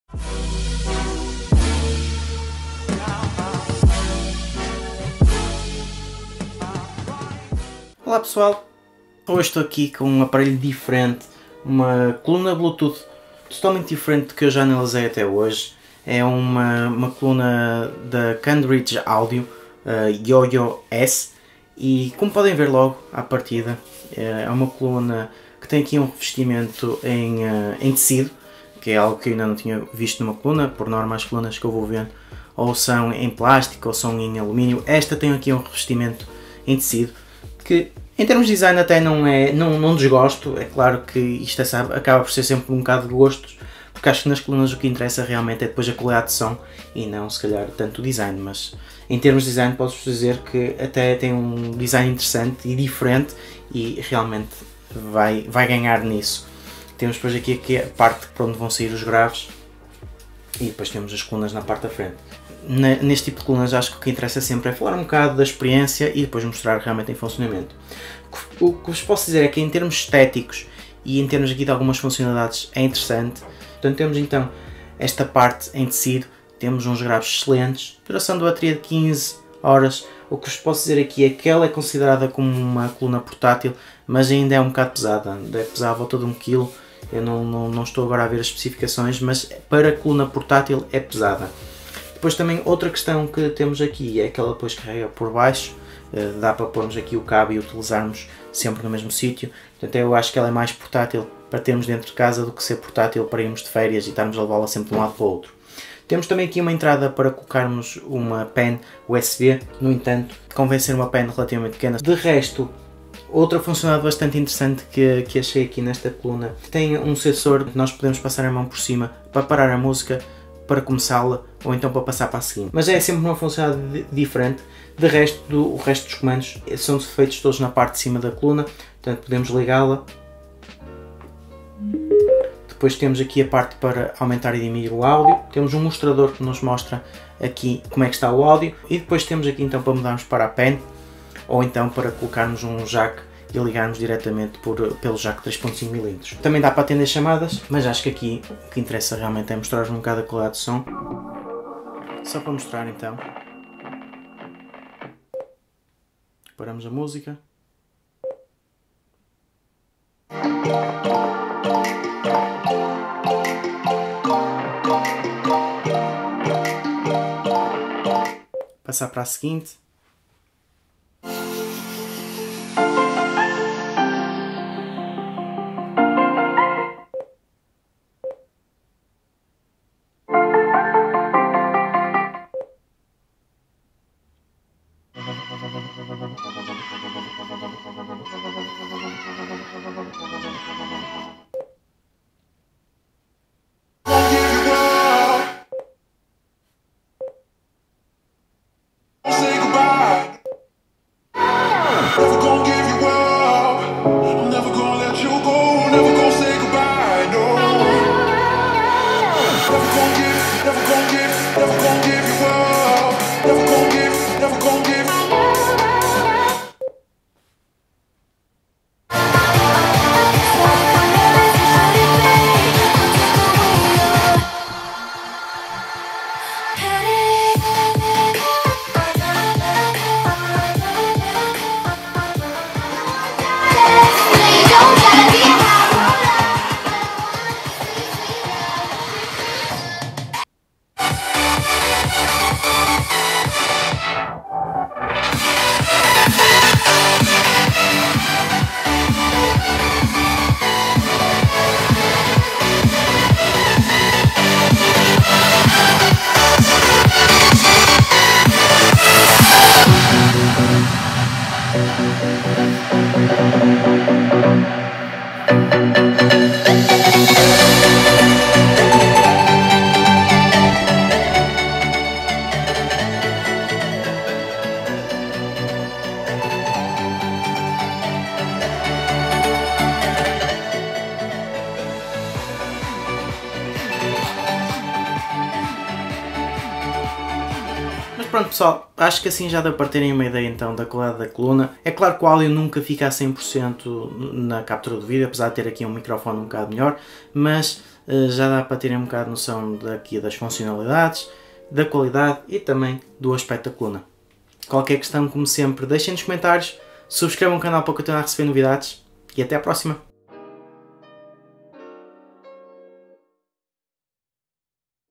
Esta controla-se através de GESTOS! | Coluna Bluetooth Cambridge Audio Yoyo (S)
Cá está uma coluna bluetooth um pouco diferente do que é habitual ver pelo canal, além de ser revestida com tecido tem controlo por gestos.